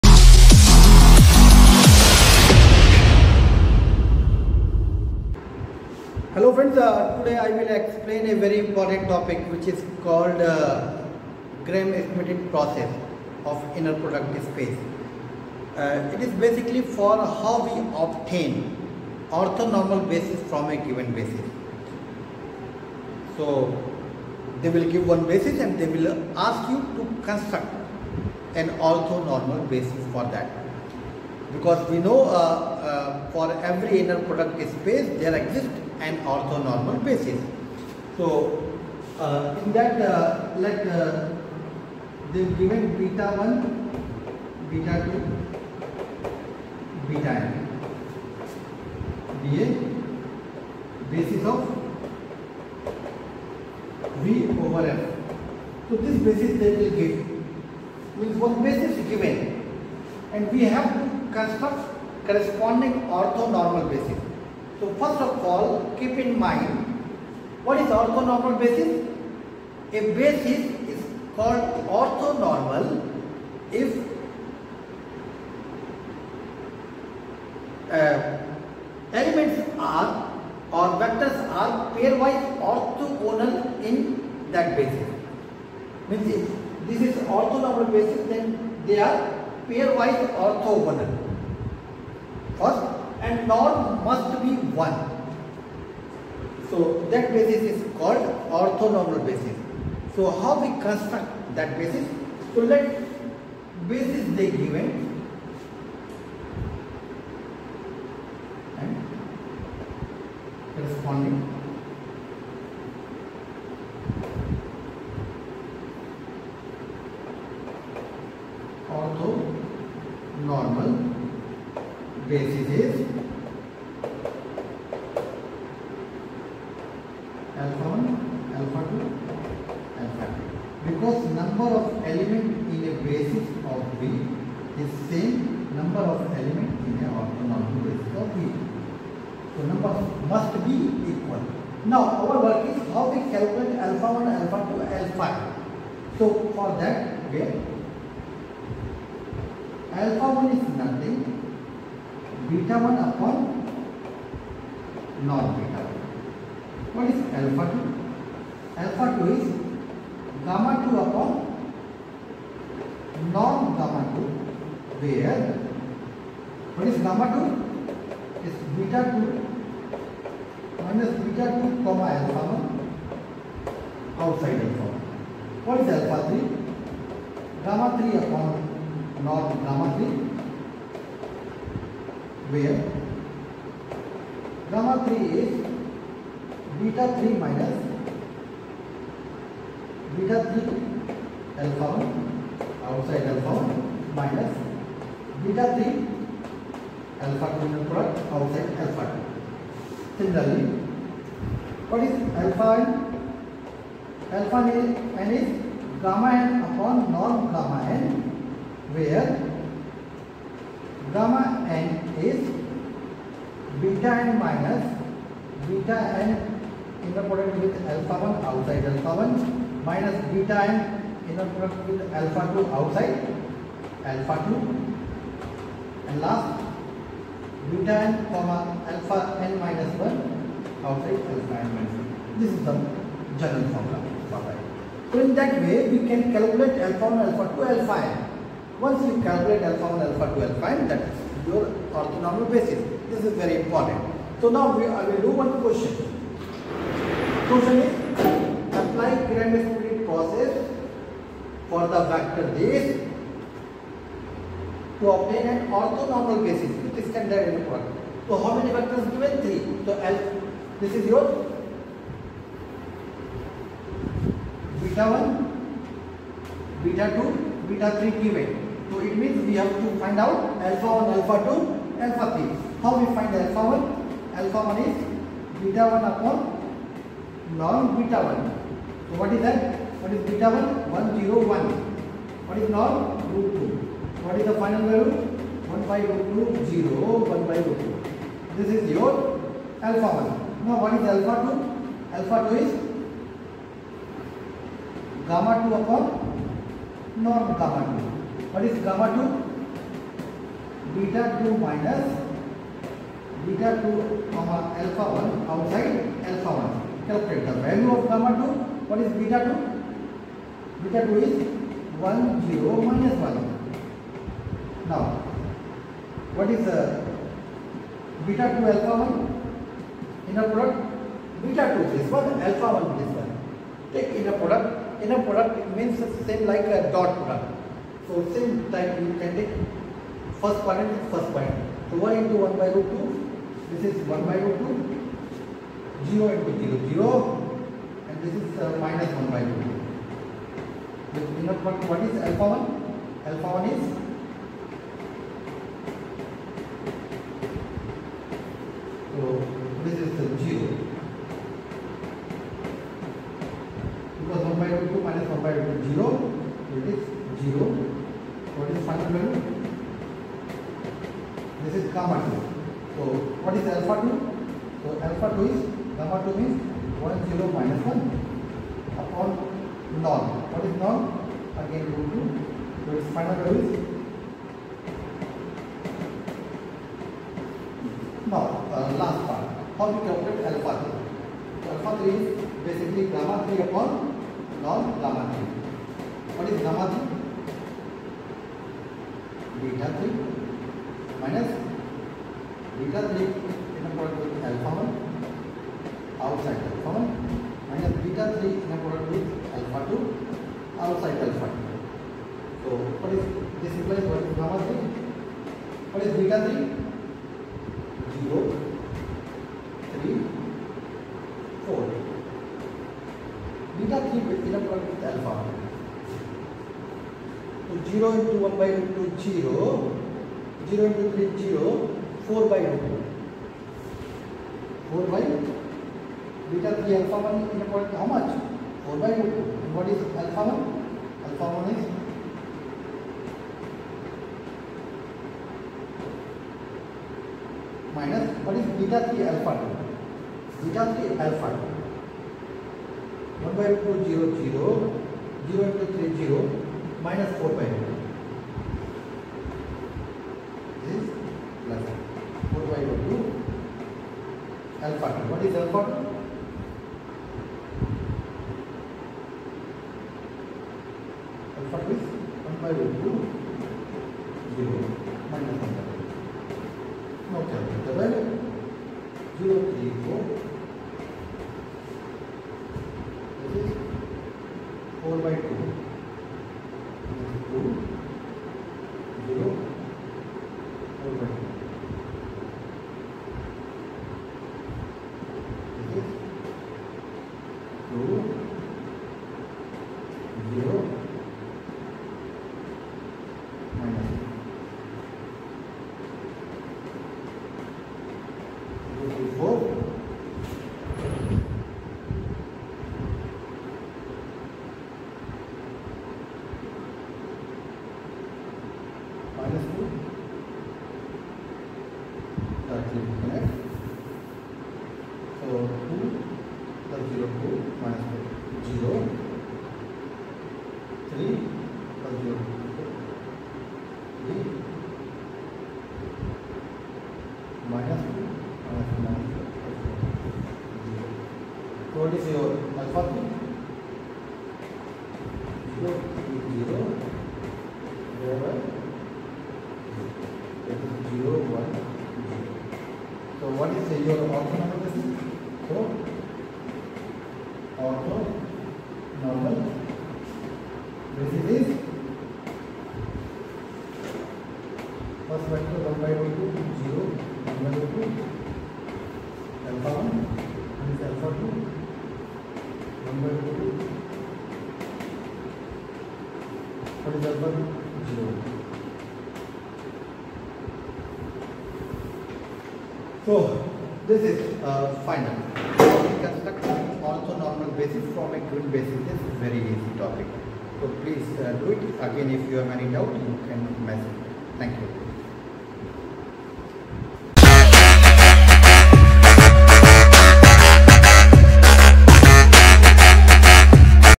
Gram-Schmidt process for orthonormal basis
Here we present to you our  Lecture on Gram-Schmidt process for orthonormal basis
The video will help you to ace competitive Exams like CSIR, GATE, and  Other Examinations.
We hope you'll like the video.

Dr. Pankaj Shukla is a Professor at the Vellore Institute of Technology.